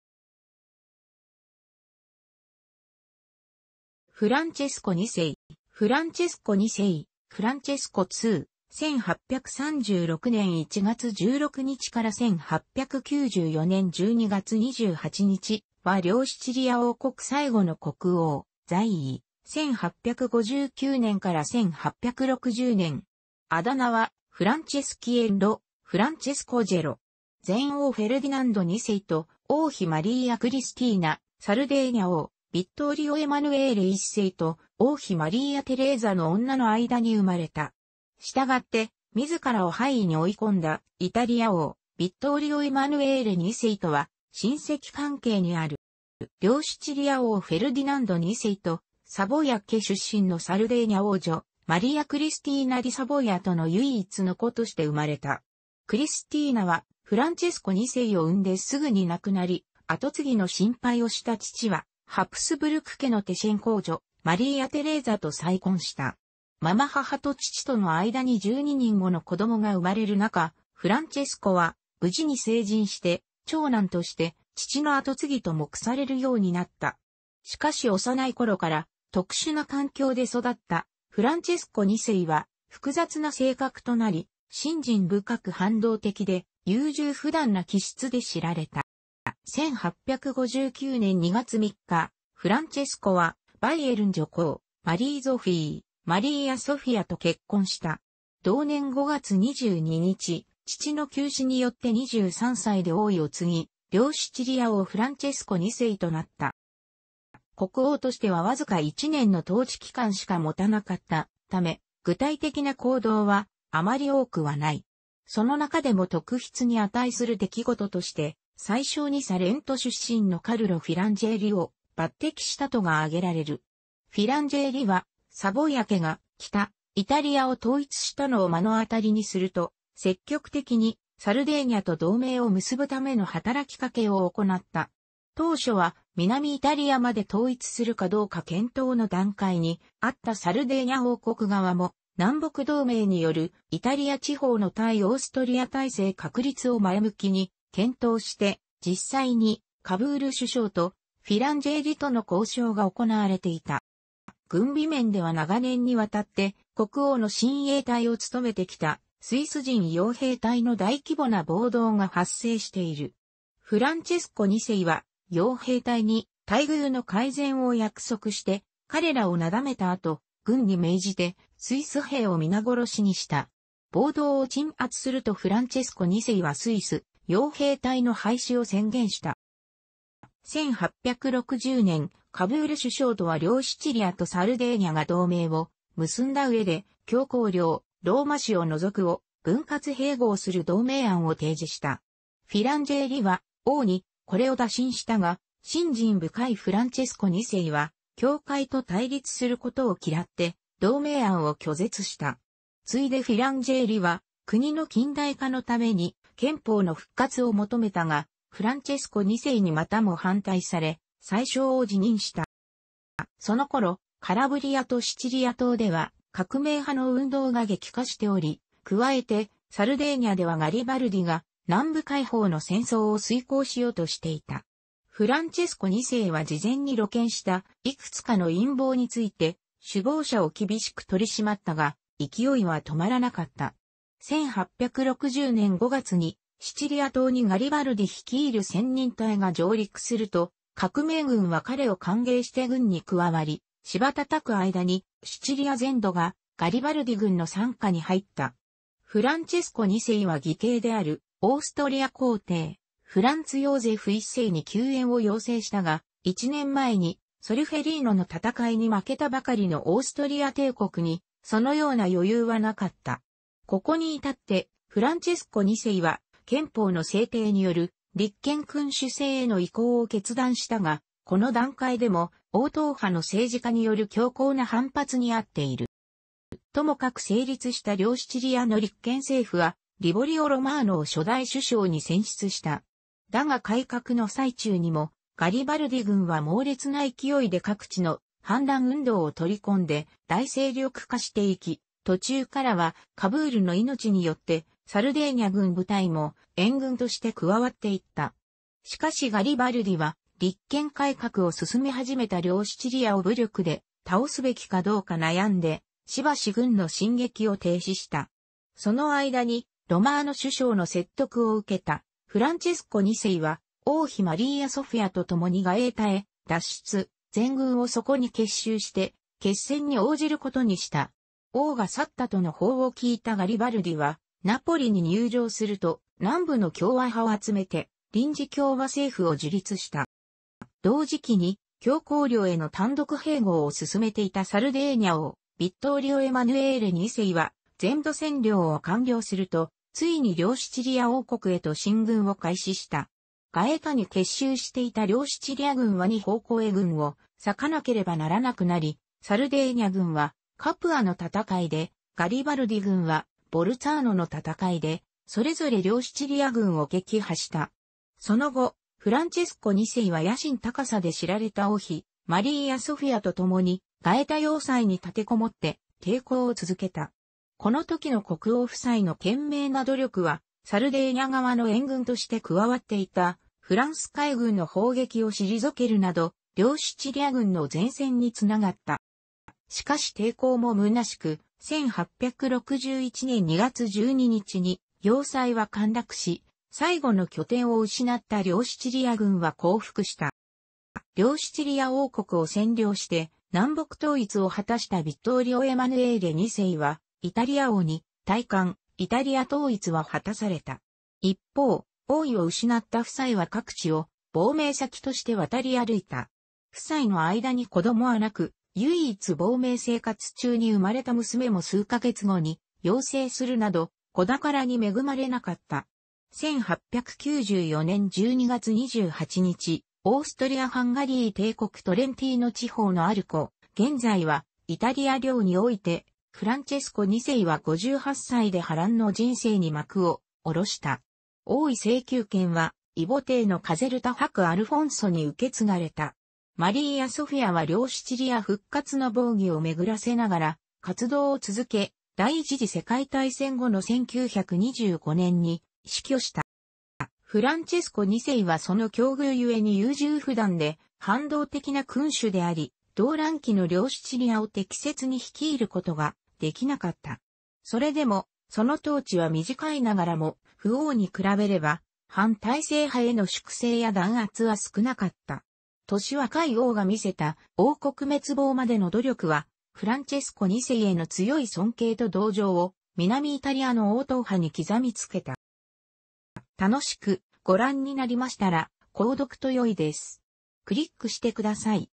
フランチェスコ2世 (両シチリア王)
ブルボン＝シチリア家

フランチェスコ2世（Francesco II, 1836年1月16日 - 1894年12月28日）は両シチリア王国最後の国王（在位：1859年 - 1860年）。あだ名はフランチェスキエッロ（Franceschiello）。前王フェルディナンド2世と王妃マリーア・クリスティーナ（サルデーニャ王ヴィットーリオ・エマヌエーレ1世と王妃マリーア・テレーザの王女）の間に生まれた。従って自らを廃位に追い込んだイタリア王ヴィットーリオ・エマヌエーレ2世とは親戚関係にある。

両シチリア王フェルディナンド2世と、サヴォイア家出身のサルデーニャ王女マリーア・クリスティーナ・ディ・サヴォイアとの唯一の子として生まれた。クリスティーナはフランチェスコ2世を生んですぐに亡くなり、跡継ぎの心配をした父はハプスブルク家のテシェン公女マリーア・テレーザと再婚した。継母と父との間に12人もの子供が生まれる中、フランチェスコは無事に成人して長男として父の跡継ぎと目されるようになった。しかし幼い頃から特殊な環境で育ったフランチェスコ2世は複雑な性格となり、信心深く反動的で、優柔不断な気質で知られた。

1859年2月3日、フランチェスコはバイエルン女公マリー・ゾフィー（マリーア・ソフィア）と結婚した。同年5月22日、父の急死によって23歳で王位を継ぎ、両シチリア王フランチェスコ2世となった。

国王としてはわずか1年の統治期間しか持たなかったため、具体的な行動はあまり多くはない。その中でも特筆に値する出来事として、宰相にサレント出身のカルロ・フィランジェーリを抜擢したことが挙げられる。フィランジェーリはサヴォイア家が北イタリアを統一したのを目の当たりにすると、積極的にサルデーニャと同盟を結ぶための働きかけを行った。当初は南イタリアまで統一するかどうか検討の段階にあったサルデーニャ王国側も、南北同盟によるイタリア地方の対オーストリア体制確立を前向きに検討して、実際にカヴール首相とフィランジェーリとの交渉が行われていた。

軍備面では長年にわたって国王の親衛隊を務めてきたスイス人傭兵隊の大規模な暴動が発生している。フランチェスコ2世は傭兵隊に待遇の改善を約束して彼らをなだめた後、軍に命じてスイス兵を皆殺しにした。暴動を鎮圧するとフランチェスコ2世はスイス傭兵隊の廃止を宣言した。

1860年、カヴール首相とは両シチリアとサルデーニャが同盟を結んだ上で教皇領（ローマ市を除く）を分割併合する同盟案を提示した。フィランジェーリは王にこれを打診したが、信心深いフランチェスコ2世は教会と対立することを嫌って同盟案を拒絶した。次いでフィランジェーリは国の近代化のために憲法の復活を求めたが、フランチェスコ2世にまたも反対され、宰相を辞任した。

その頃、カラブリアとシチリア島では革命派の運動が激化しており、加えてサルデーニャではガリバルディが南部解放の戦争を遂行しようとしていた。フランチェスコ2世は事前に露見した幾つかの陰謀について首謀者を厳しく取り締まったが、勢いは停まらなかった。1860年5月にシチリア島にガリバルディ率いる千人隊が上陸すると、革命軍は彼を歓迎して軍に加わり、瞬く間にシチリア全土がガリバルディ軍の傘下に入った。フランチェスコ2世は義兄であるオーストリア皇帝フランツ・ヨーゼフ1世に救援を要請したが、1年前にソルフェリーノの戦いに負けたばかりのオーストリア帝国にそのような余裕はなかった。ここに至ってフランチェスコ2世は憲法の制定による立憲君主制への移行を決断したが、この段階でも王党派の政治家による強硬な反発に遭っている。

ともかく成立した両シチリアの立憲政府は、リボリオ・ロマーノを初代首相に選出した。だが改革の最中にもガリバルディ軍は猛烈な勢いで各地の反乱運動を取り込んで大勢力化していき、途中からはカヴールの命によってサルデーニャ軍部隊も援軍として加わっていった。しかしガリバルディは、立憲改革を進め始めた両シチリアを武力で倒すべきかどうか悩んで、しばし軍の進撃を停止した。その間にロマーノ首相の説得を受けたフランチェスコ2世は、王妃マリーア・ソフィアと共にガエータへ脱出、全軍をそこに結集して決戦に応じることにした。王が去ったとの報を聞いたガリバルディはナポリに入城すると、南部の共和派を集めて臨時...